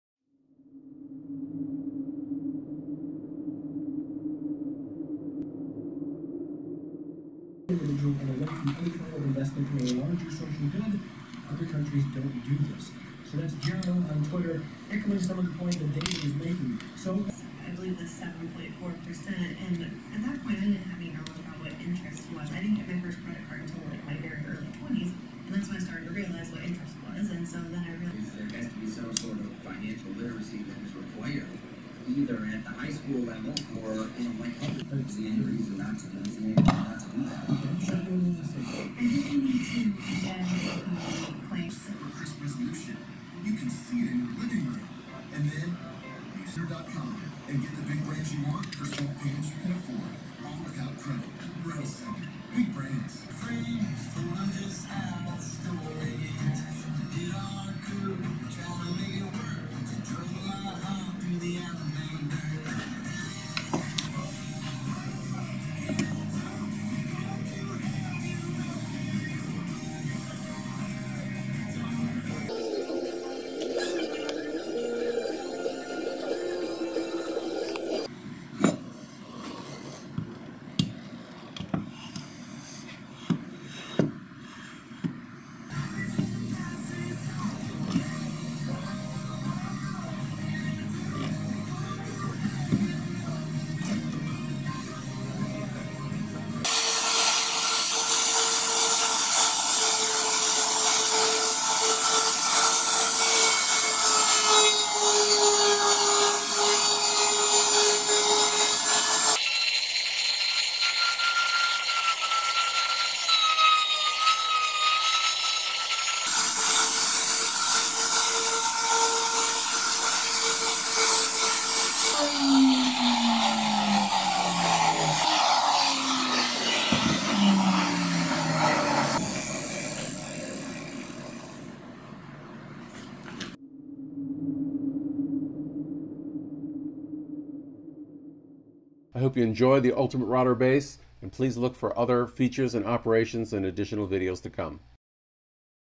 Mortising with Self-Centering Guide Pins
Using self-centering guide pins to cut a mortise with The Ultimate Router Base.
Beta Testers Wanted